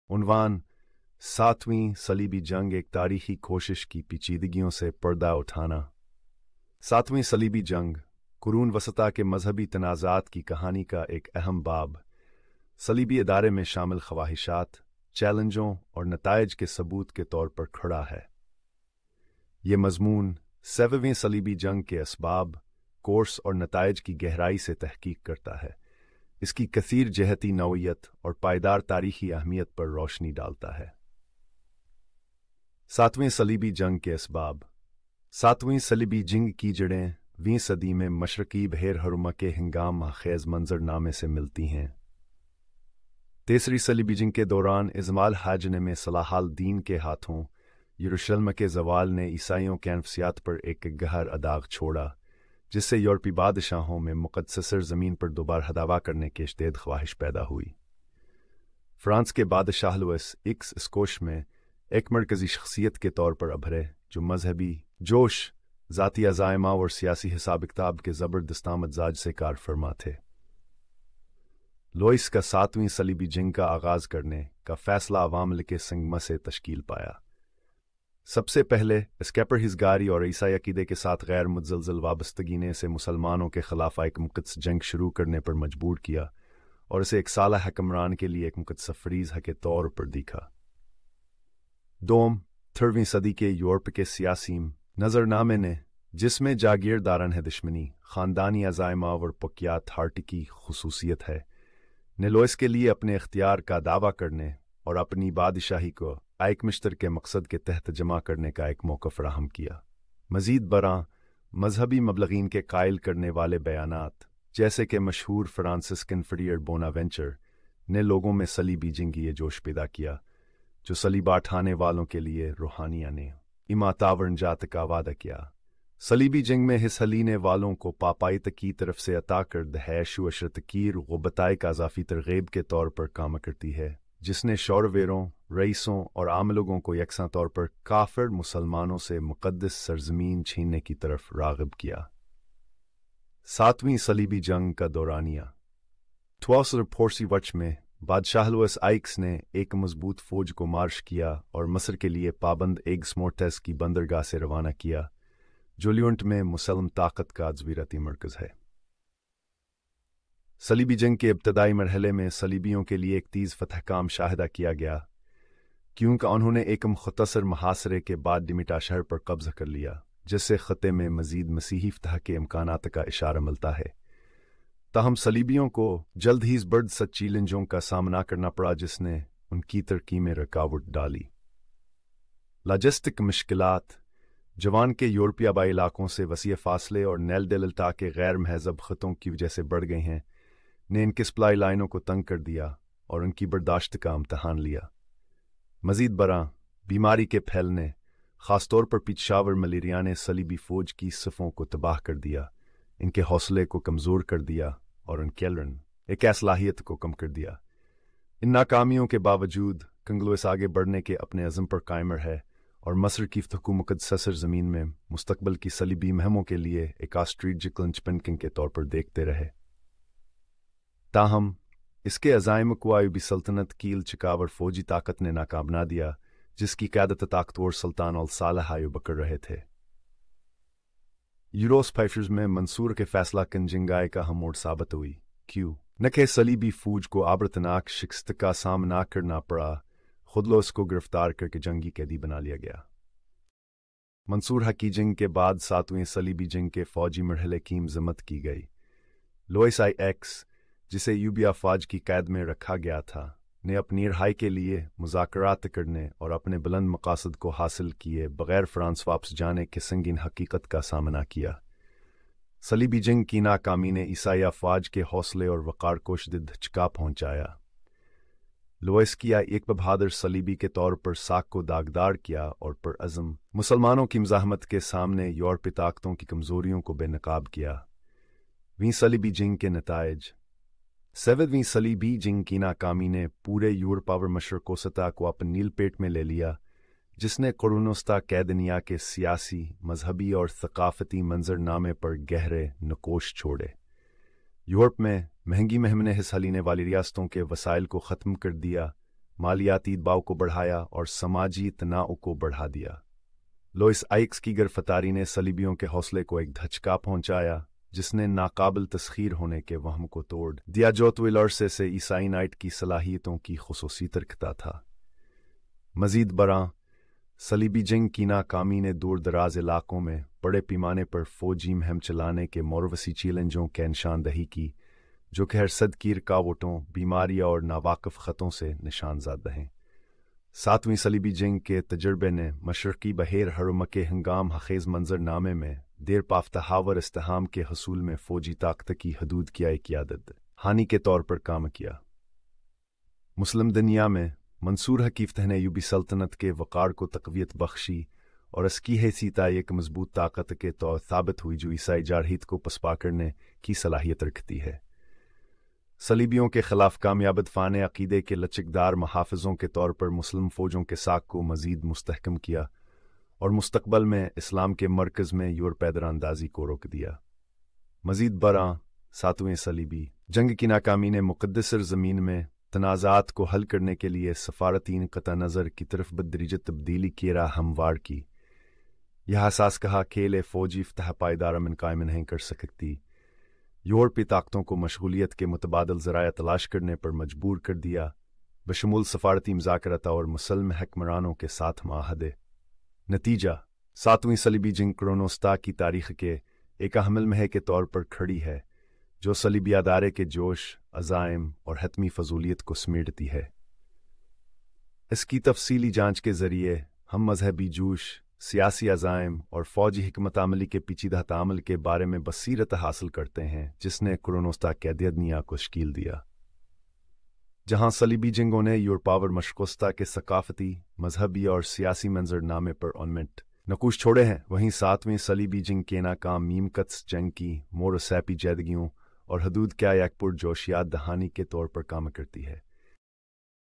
فلسطین-اسرائیل تنازع کی سیری:آٹھویں صلیبی جنگ: سمندروں سے آگے فتح کے لیے جدوجہد
): قرون وسطی کی جنگ اور سفارت کاری کے دائرے میں قدم رکھیں جب ہم آٹھویں صلیبی جنگ کی کہانی سے پردہ اٹھاتے ہیں، جو سمندروں سے آگے فتح حاصل کرنے کی ایک جرات مندانہ کوشش ہے۔ فرانس کے بادشاہ لوئس IX کے نقش قدم پر چلیں جب وہ عیسائی طاقتوں کو شمالی افریقہ کے مرکز میں لے جاتا ہے، عیسائی تسلط کو بڑھانے کی کوشش میں مضبوط مسلم طاقتوں کا مقابلہ کرتا ہے۔ دور دراز ممالک میں صلیبی جنگ کے چیلنجوں کو روشن کرتے ہوئے اس تاریخی مہم کے محرکات، آزمائشوں اور فتنوں کو دریافت کریں۔